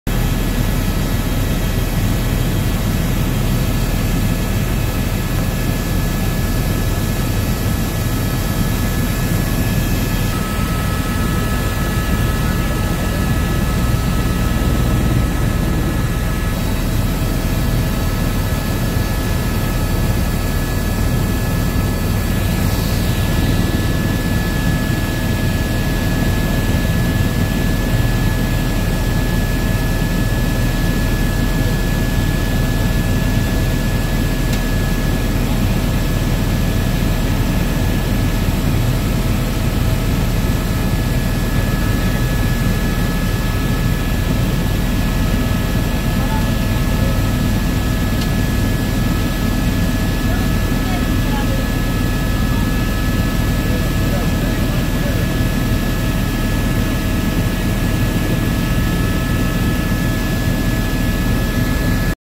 BLUE HAWAIIAN HELICOPTERS (HOLO HOLO TOUR) BIG ISLAND, HAWAII PART 9/11
FEBRUARY 2020:BLUE HAWAIIAN HELICOPTERS (HOLO HOLO TOUR) BIG ISLAND, HAWAII.